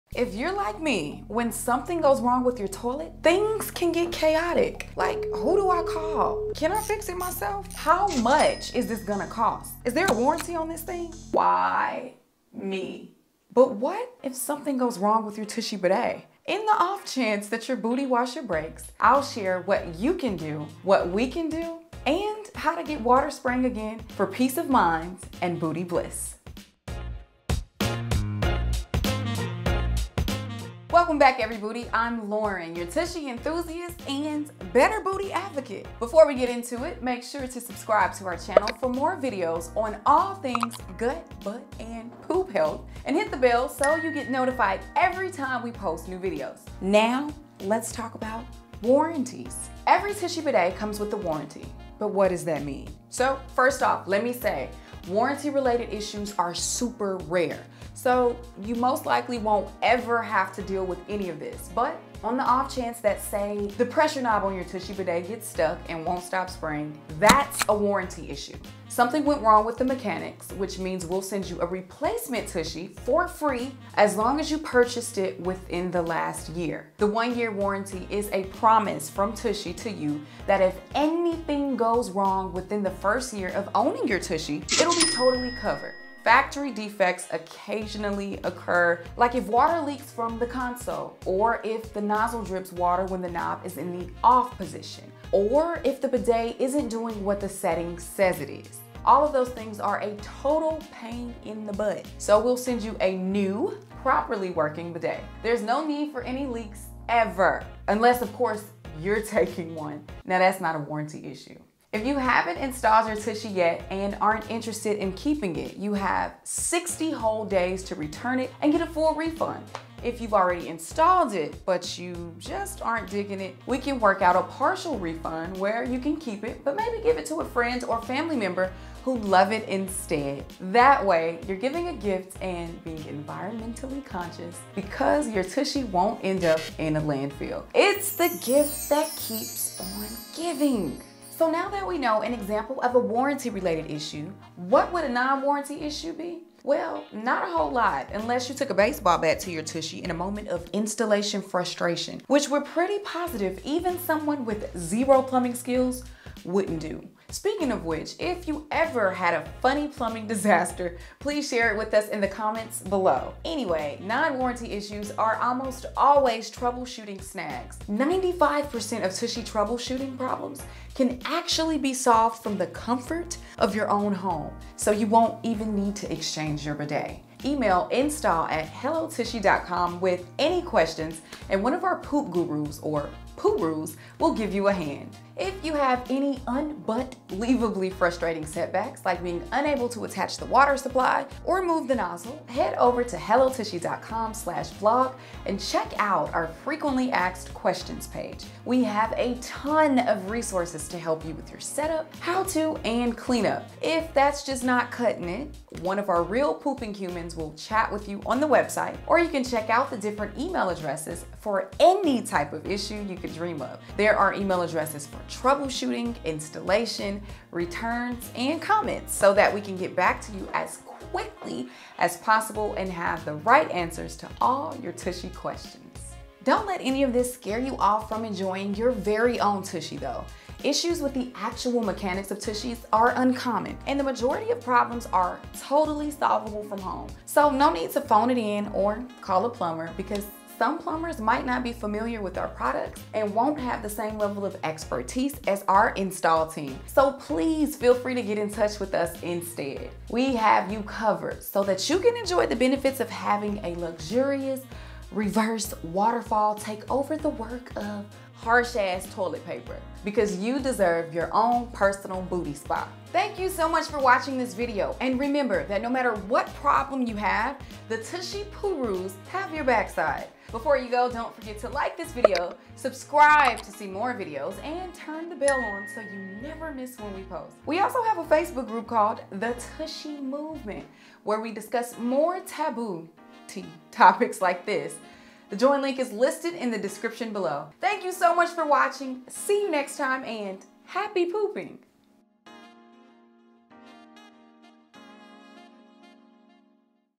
TUSHY Bidet Warranty and What To Do If Something Breaks (2021)
In the uncommon, but unfortunate scenario that your TUSHY bidet isn’t working the way it’s supposed to, stick around to find out your TUSHY bidet warranty options, understand troubleshooting issues, and how to get back to booty blasting bliss!

Timestamps to help guide you:
00:00 Intro
00:51 TUSHY Bidet Warranty 101
02:23 Non-Warranty Issues
02:50 Installation and Troubleshooting Support
03:48 Use your TUSHY with Ease!

To learn more about the TUSHY bidet and our other products